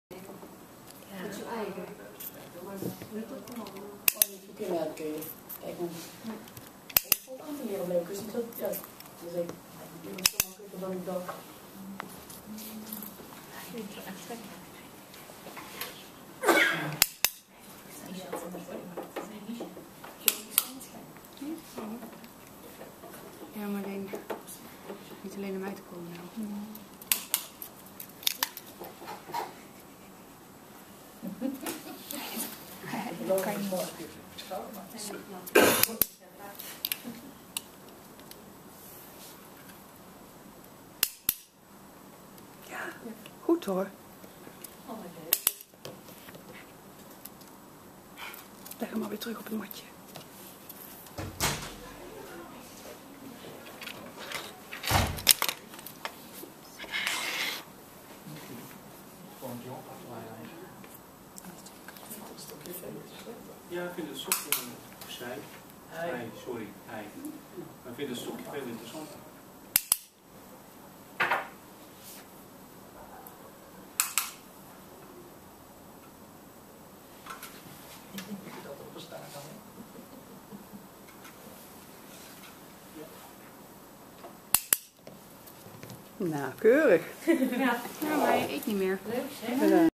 Opleiding Dierentrainer Targettraining cavia voetballen
Impressie van de opleiding Dierentrainer van Cursuscentrum Dierverzorging in Barneveld. Aan bod komen uitgebreid de verschillende leervormen. Daarnaast leren de cursisten zelfstandig trainingen op te zetten en uit te voeren.